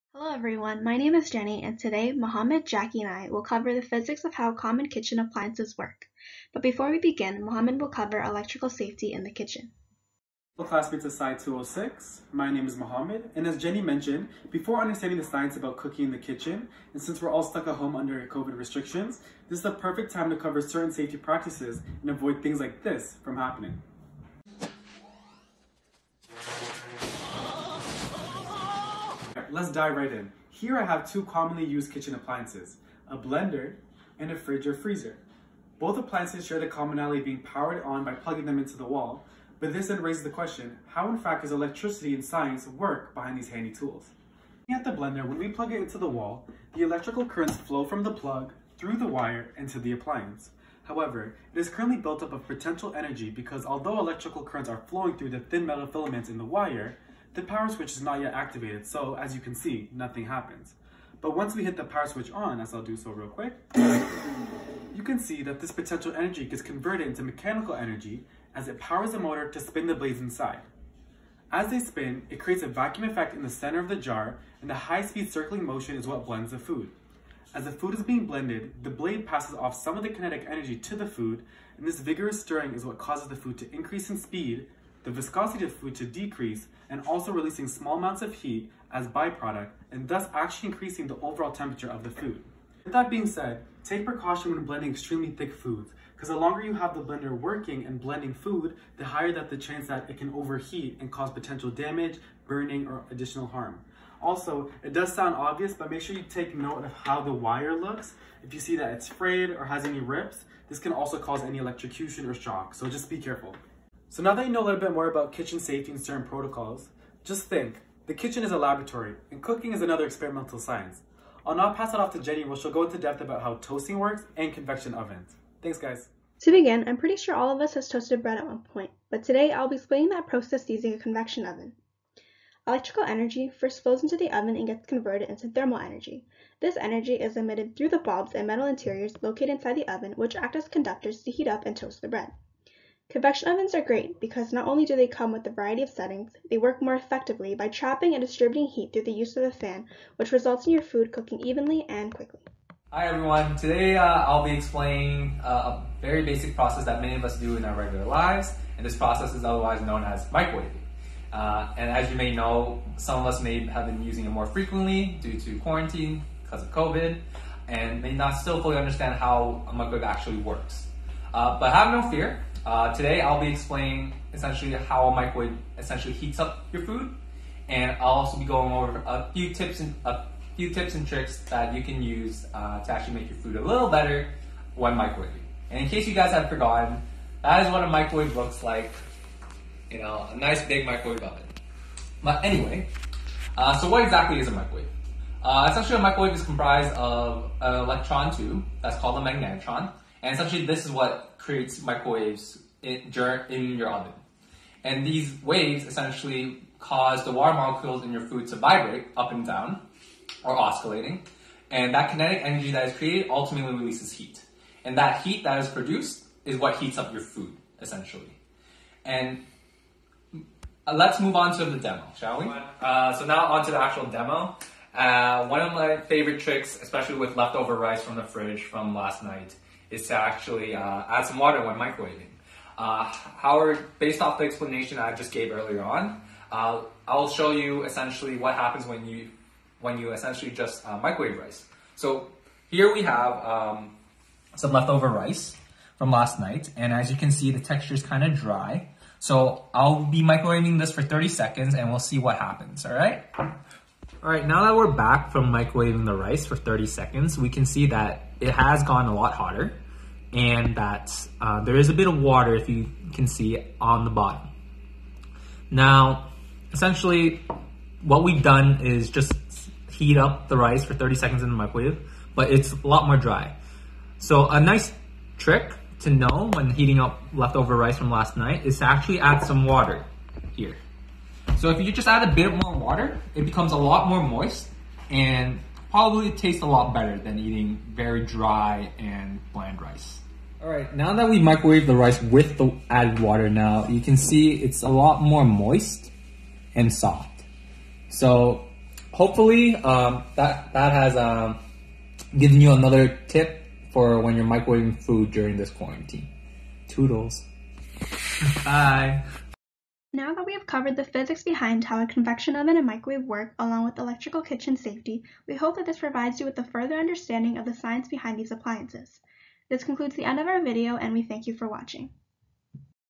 SCI206: The Physics of How Common Kitchen Appliances Work
Created by:
Mohamed Hajjo
Jenny Dang
Jacky Xiao

Edited by:
Mohamed Hajjo

References used (informational content):
- Barham, P. Physics in the kitchen. Flavour 2, 5 (2013).

- Health, Center for Devices and Radiological. “Microwave Oven Radiation.” FDA, 9 Feb.

- Everything You Need to Know About Convection Toaster Ovens.” Toaster Oven Love, 30 Oct. Accessed 23 Nov. 2020